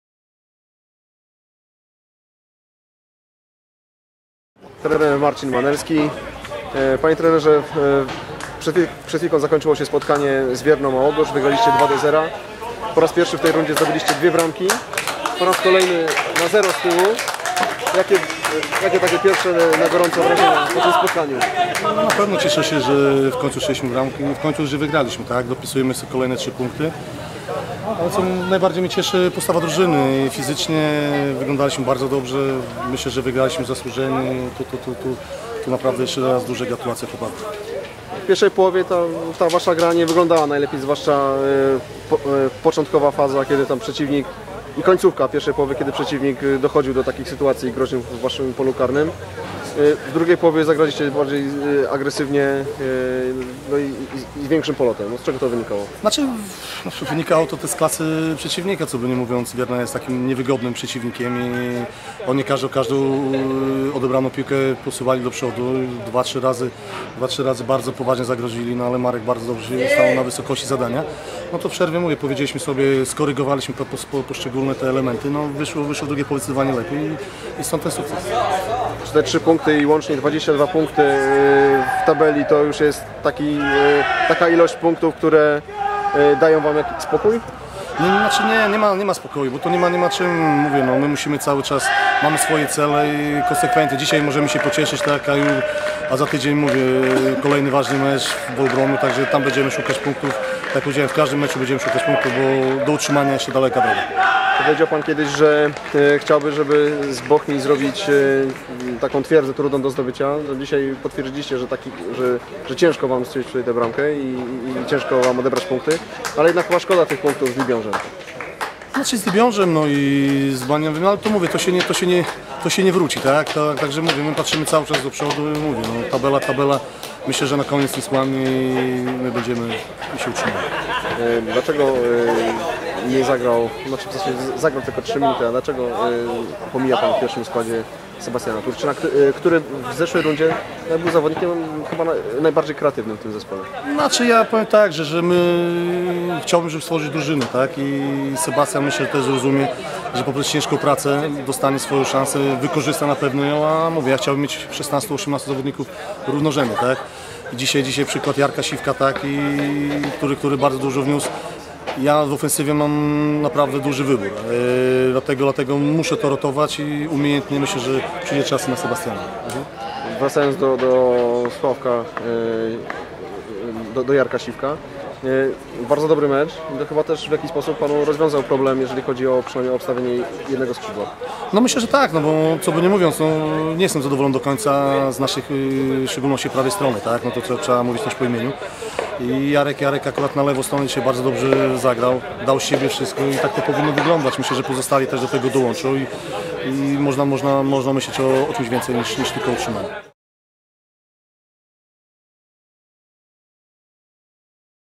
Marcin Manelski podsumowuje wygrany mecz BKS-u z Wierną
Bardzo zadowolony ze zwycięstwa, trzech punktów i pierwszych bramek na własnym stadionie w rundzie wiosennej był Marcin Manelski - opiekun BKS-u. Oto co szkoleniowiec miał do powiedzenia po tym spotkaniu